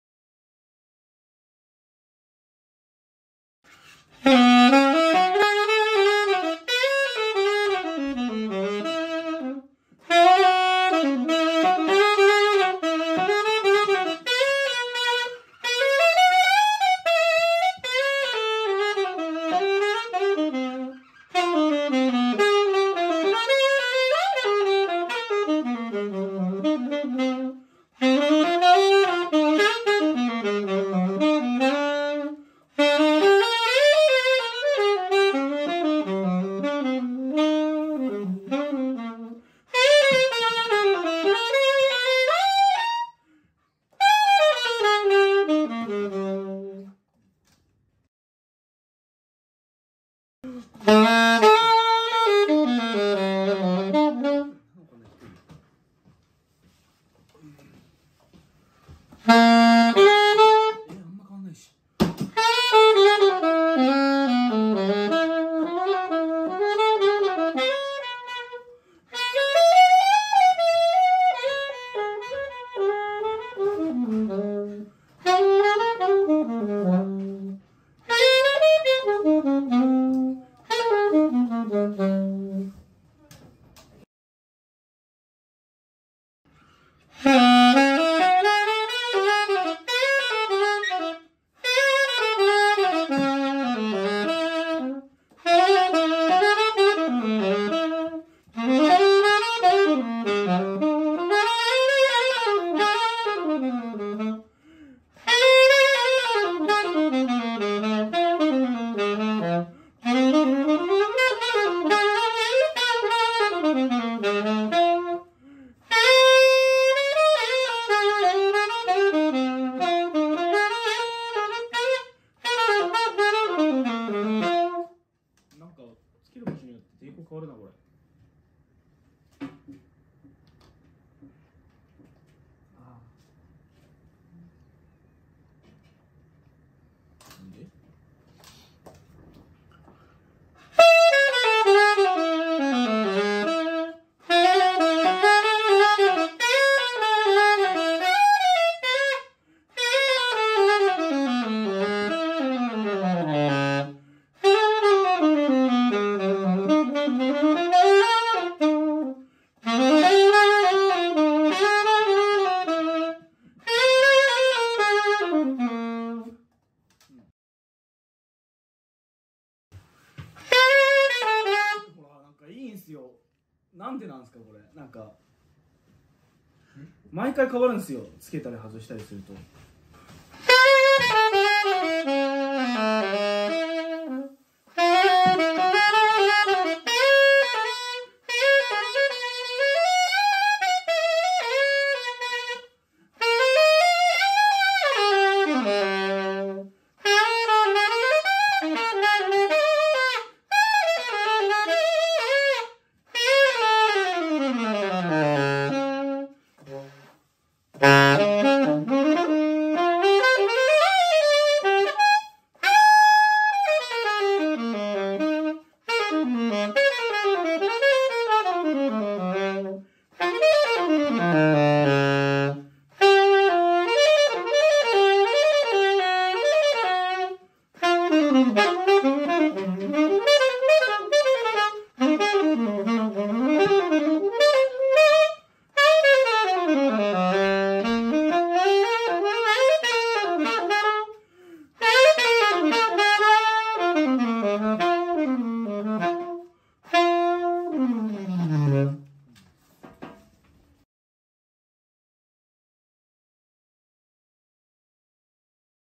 クラシックで評判がいいNymphリガチャーを佐藤大雪氏が試奏！
クラシックで評価が高いNymphリガチャーをバンドサポートやレコーディングで引っ張りだこの佐藤大雪氏に試奏していただきました。

同時上映
松下洋氏の鏡ふきふき

大きく立派な鏡を磨いている松下氏の後ろ姿、なぜかずっと見ていられます。